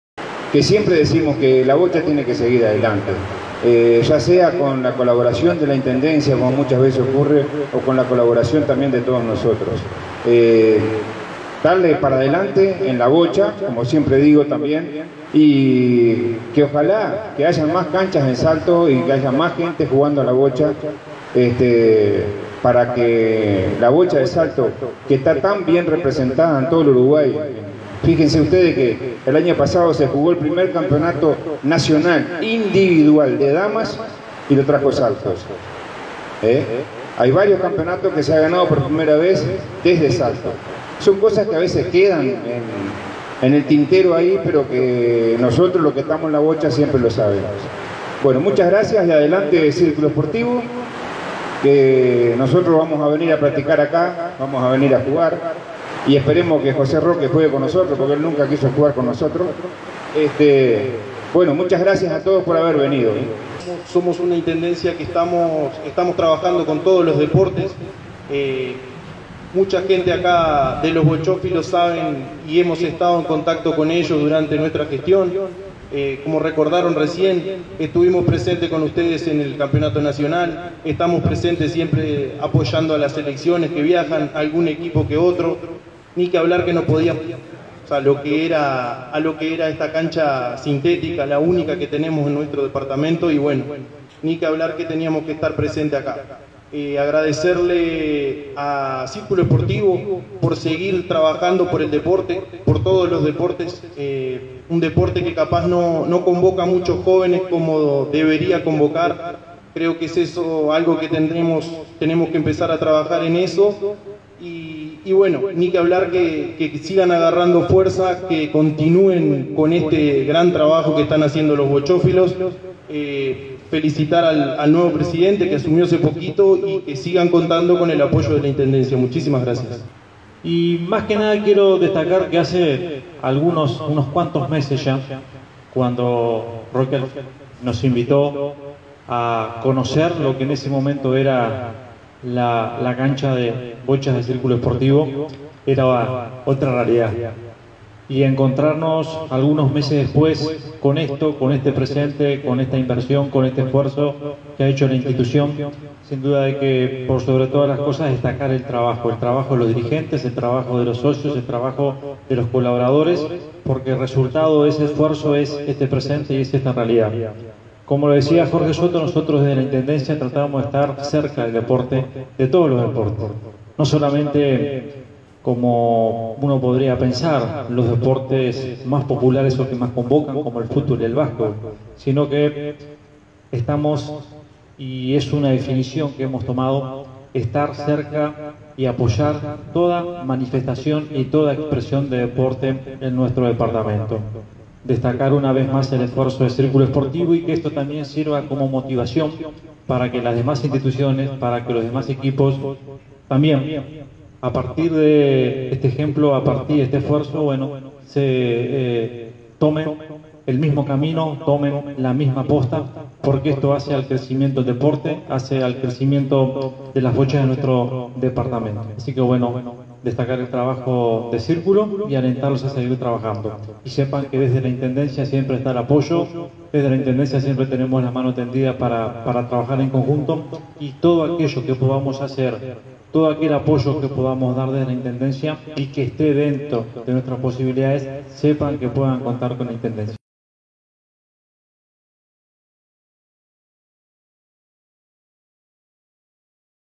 Círculo Sportivo inauguró cancha de bocha sintética
Con la presencia de autoridades departamentales y del deporte, se inauguró en Círculo Sportivo una cancha sintética de bochas...